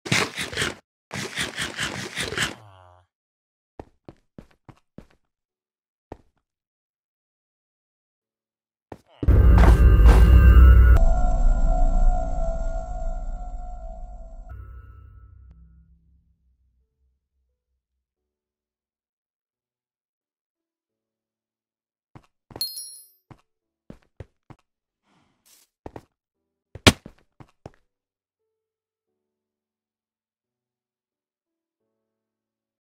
Void vs Snow Golem Boss
Normally you trap the snow golem boss in a 1 by 1 space but i wanted to be more creative than that!